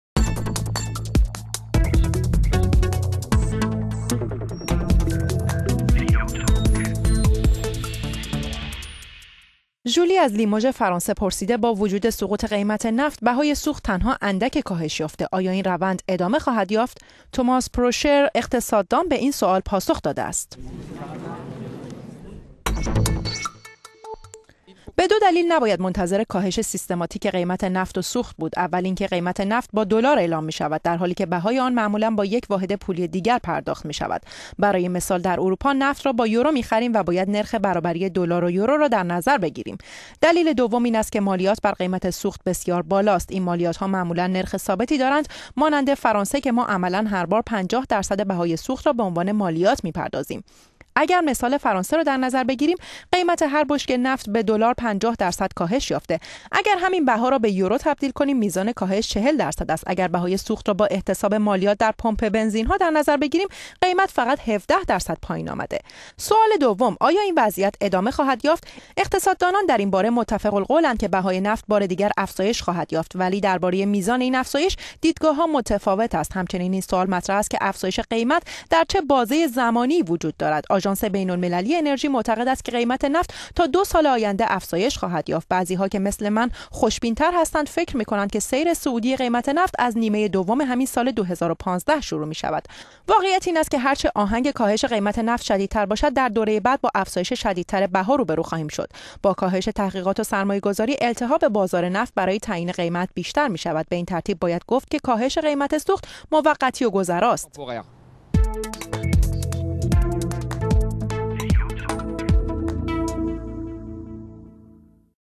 کاهش قیمت نفت و سوخت گذرا است - utalk
ژولی از لیموژ فرانسه پرسیده است : با وجود سقوط قیمت نفت، بهای سوخت تنها اندکی کاهش یافته است. آیا این روند ادامه خواهد یافت؟

توماس پورشر، اقتصاد دان به این پرسش پاسخ می دهد: «به دو دلیل نباید منتظر کاهش سیستماتیک قیمت نفت و سوخت بود. اول این که، قیمت نفت با دلار اعلام می شود، در حالی که بهای آن معمولا با یک واحد پولی دیگر پرداخت می شود. برای مثال در اروپا، نفت را با یورو می خریم و باید نرخ برابری دلار و یورو را در نظر داشت.

وی درباره تاثیر مالیات ها بر افزایش قیمت سوخت می گوید: «دلیل دوم این ا…